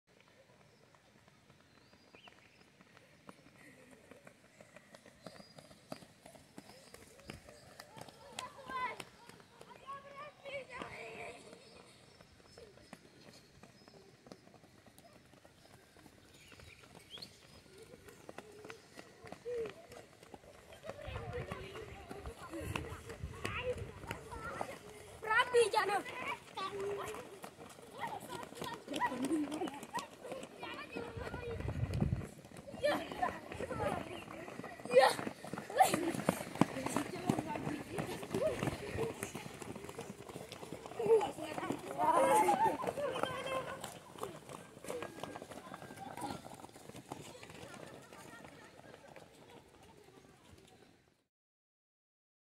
Kapchorwa, Uganda - Where Running is Life. Young Athletes from 'Home of Talents' Kids Athletics
Running is a way of life in Kapchorwa, Uganda's 'Land of Champions'. In Kapenguria, a small village ~5km from Kapchorwa Town, young runners gather to train with dreams to become the next Joshua Cheptegei, Jacob Kiplimo or Halima Nakaayi.